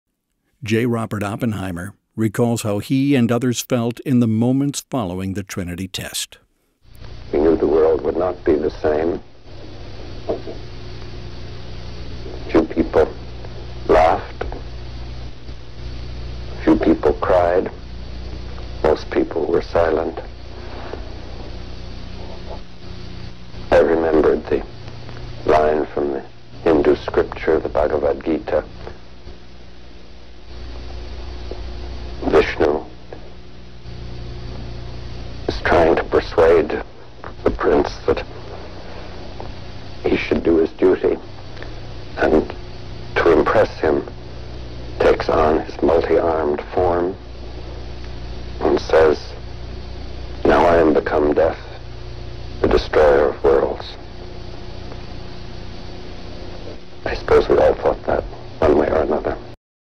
Now I Am Become Death
Los Alamos laboratory director J. Robert Oppenheimer describes his response to the Trinity Test.